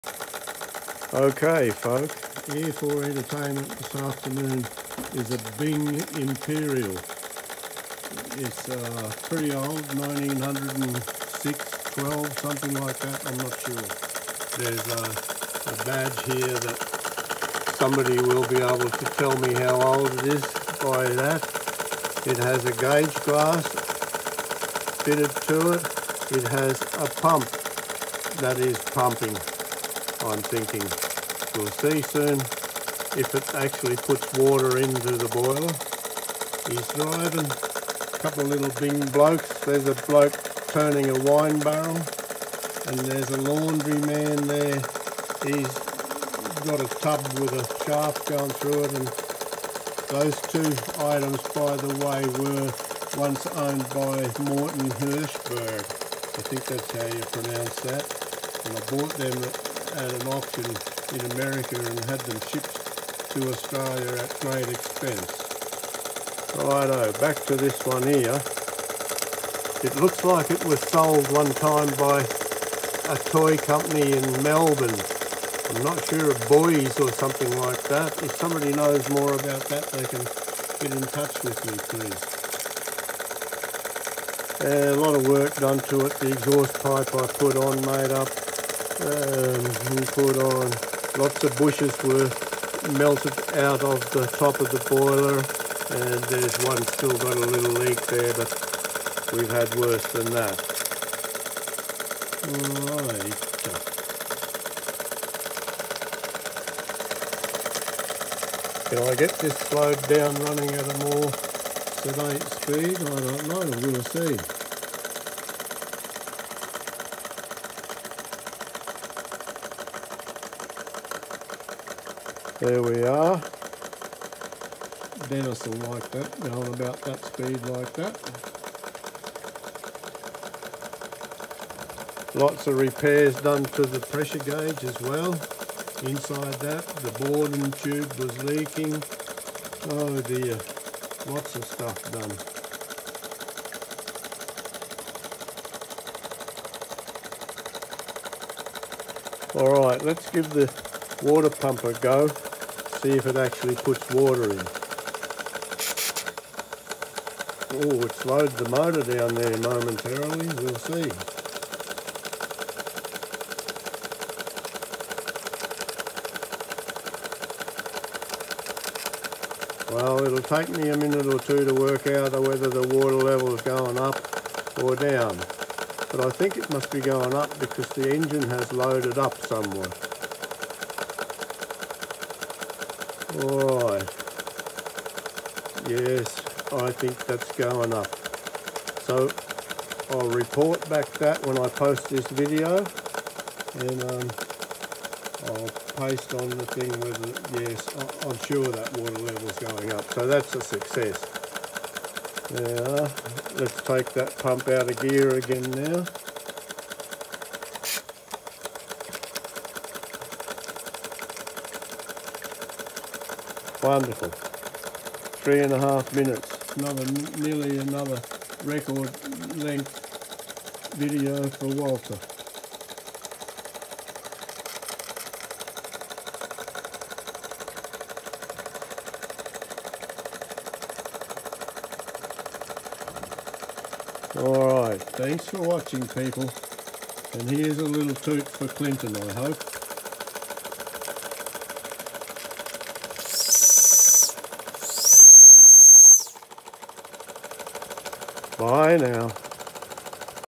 Bing vertical steam engine 8552/2 1/2
First run and demonstration after serious repair and for viewing pleasure, & Yes, the water pump works :-)